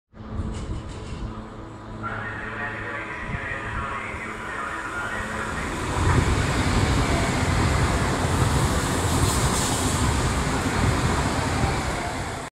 ETR610 014 SBB - Sesto San Giovanni 25/05/2015
L'ETR610 014 SBB, ultimo della prima serie ex Cisalpino, effettua l'EC 20 Milano C.le-Zurigo.